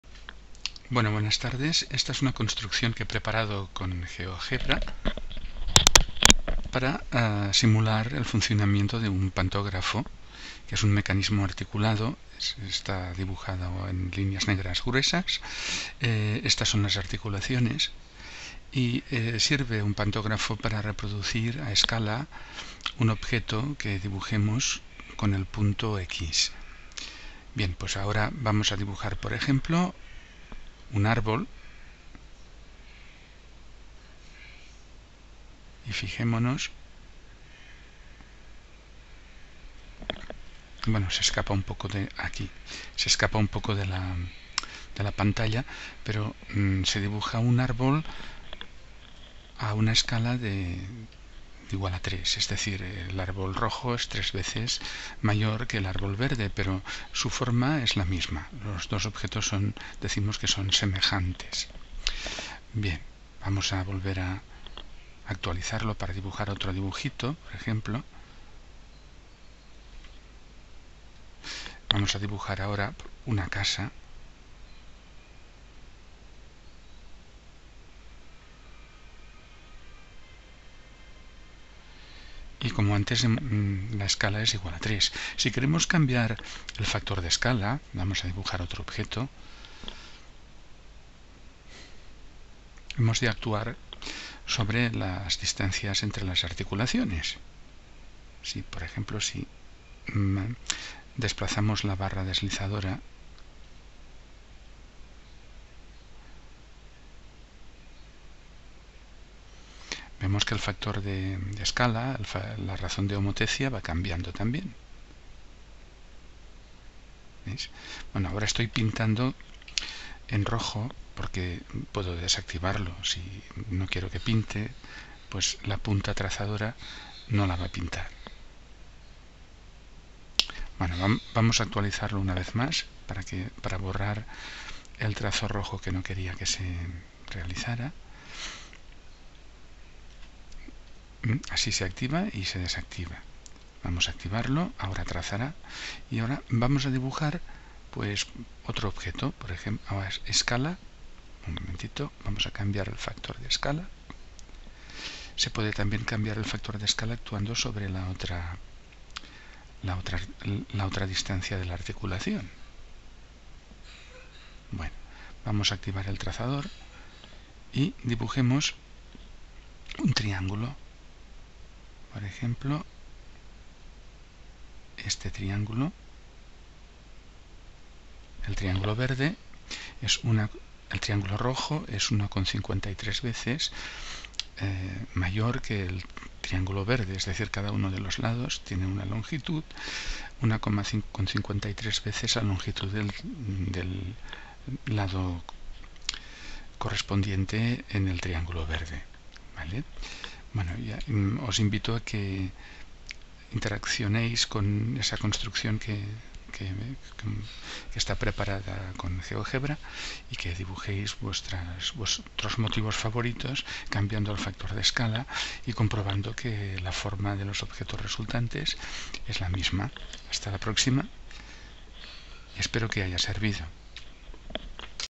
Simulación de un pantógrafo con GeoGebra
Este vídeo muestra cómo se puede dibujar un objeto a distintas escals mediane el uso de un instrumento articulado que se llama pantógrafo.